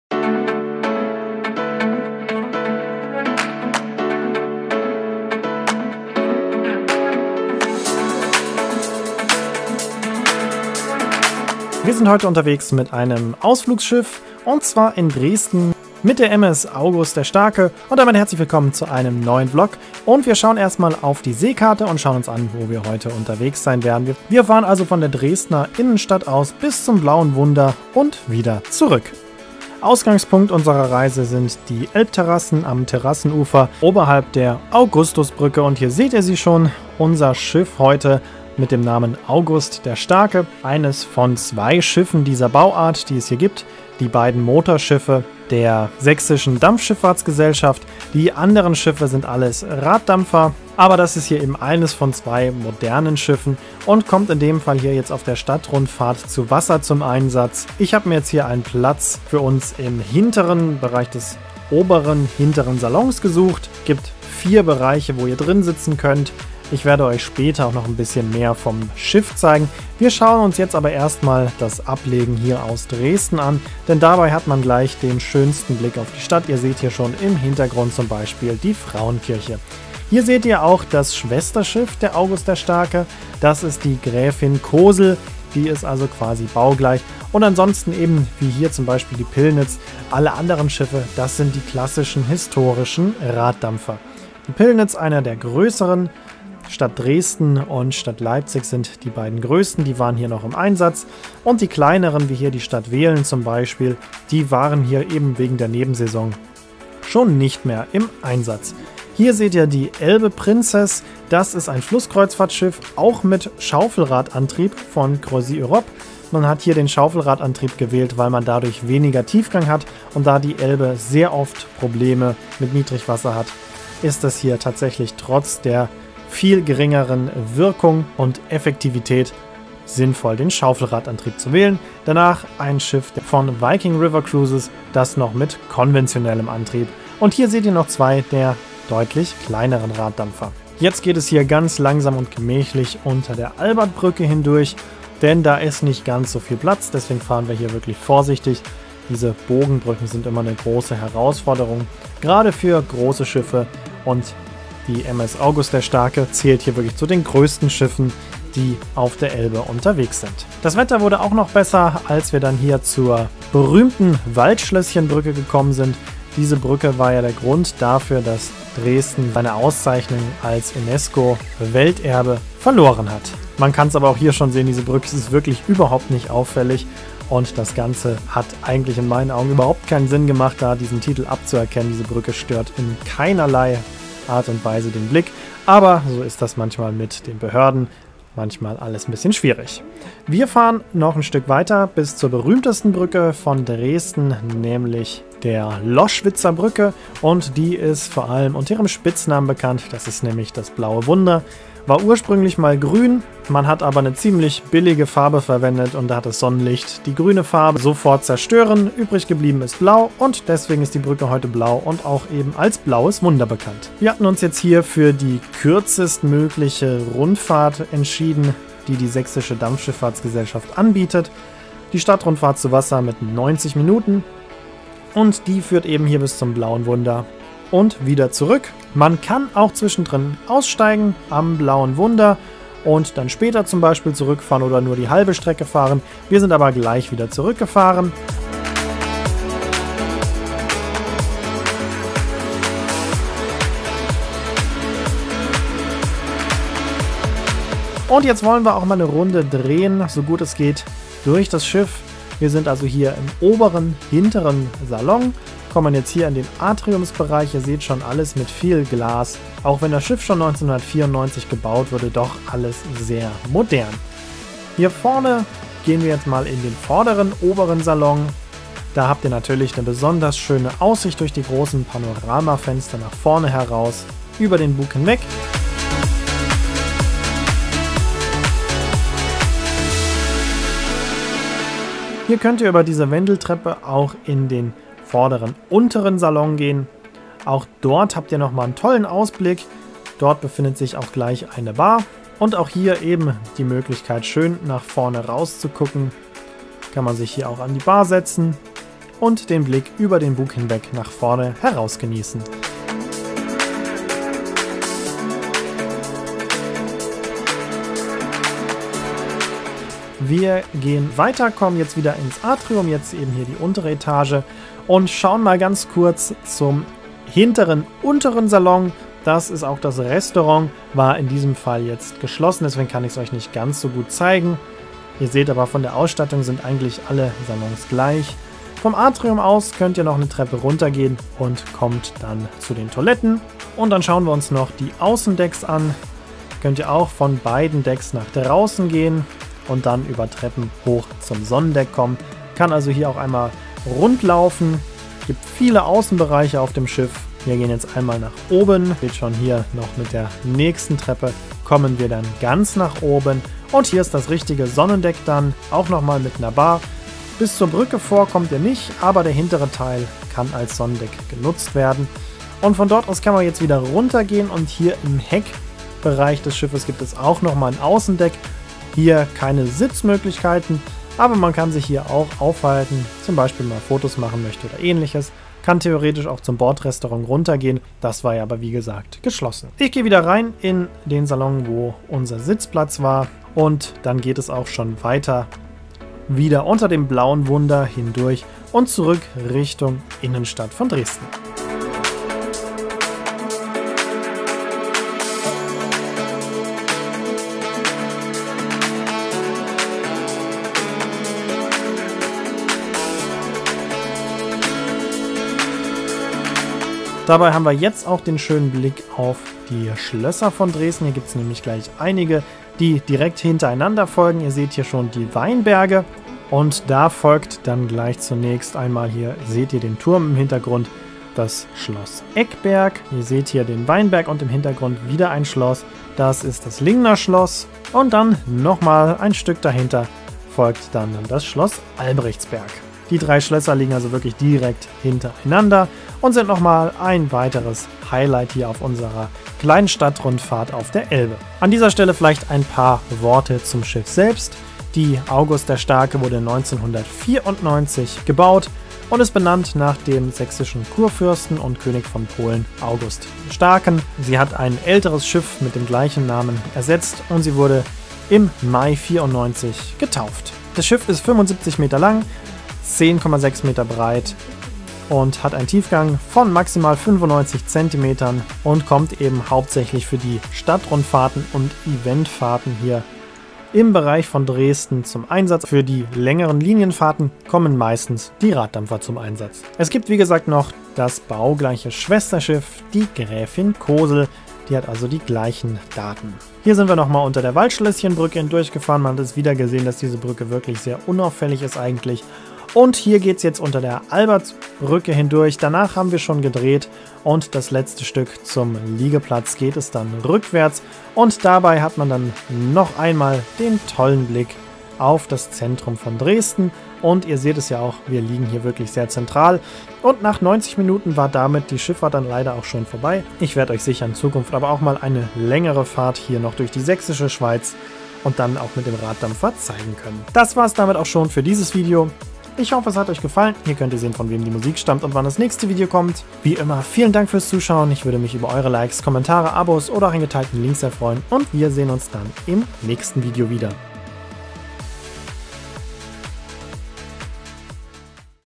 Mit der MS AUGUST DER STARKE durch Dresden | Vlog 577
Vlog 577: Die MS August der Starke ist eines von zwei modernen Schiffen der Sächsischen Dampfschifffahrtsgesellschaft und wird oft für Stadtrundfahrten eingesetzt. Wir fahren diesmal von der Innenstadt bis zum Blauen Wunder und zurück.
Aufnahmezeitpunkt: September 2021

Camera gear:
Vlogs: Sony Alpha 6400, GoPro Hero7 Black
Photography: Sony A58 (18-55; 70-300)
Edit: Adobe Premiere Pro CC 2021